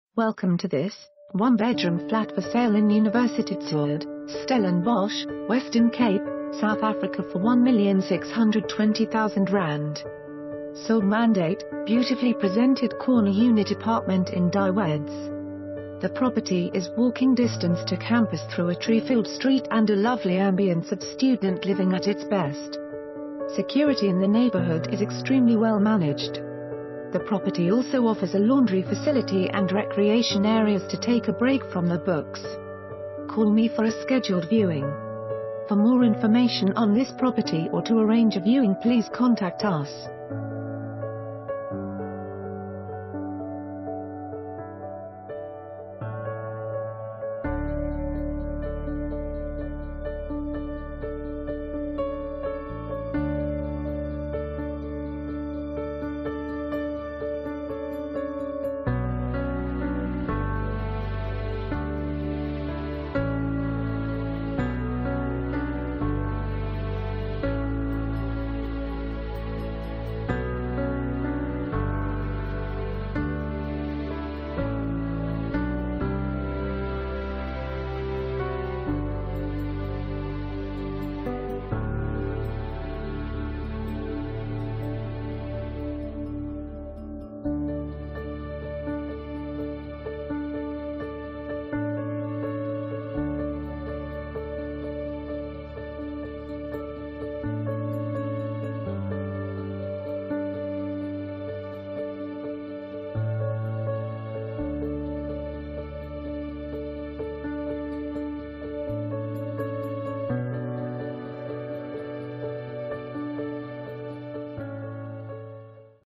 1 Bedroom Flat For Sale in Universiteitsoord, Stellenbosch, Western Cape, South Africa for ZAR 1,...
Beautifully presented corner unit apartment in Die Weides, Stellenbosch.

The apartment  is walking distance to campus  through a tree filled street and a lovely ambiance of student living at its best.

Security in the neighborhood is extremely well managed.

The property also offers a laundry facility and recreation areas...